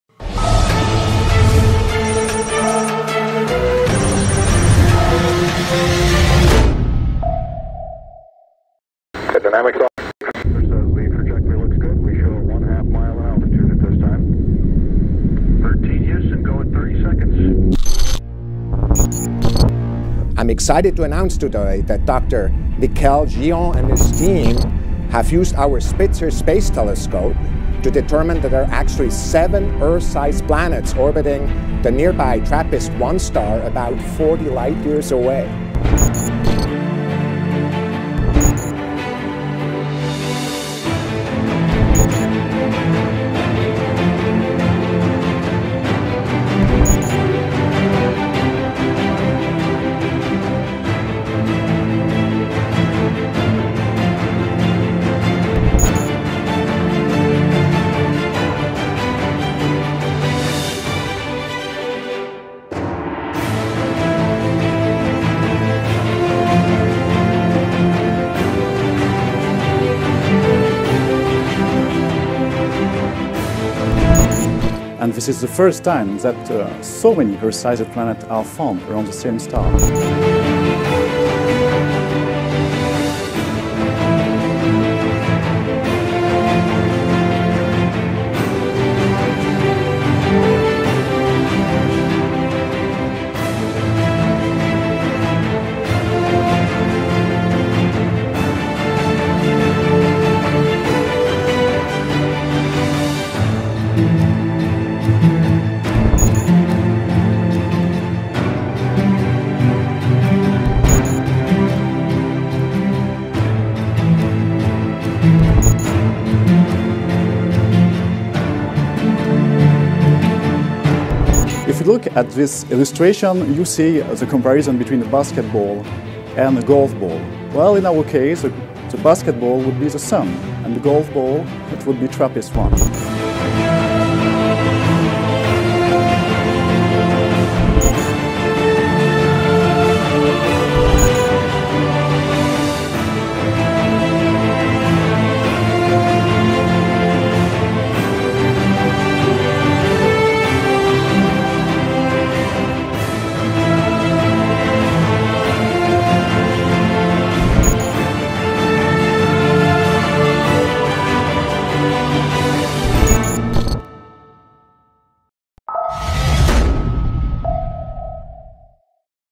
كل ما تريد أن تعرفه عن كواكب "ناسا" السبعة
لسنا وحدنا في هذا الكون، أو لسنا الكوكب الوحيد القابل للحياة، هناك سبعة كواكب أخرى مثيلة لكوكب الأرض، هكذا أخبرت وكالة "ناسا" الفضائية العالم أمس، في هذا الفيديو كل ما تريد أن تعرفه عن هذه الكواكب السبعة، كم تبعد عنا، وكم يلزمنا من الزمن للوصول إليها، عبر رحلة طائرة عادية.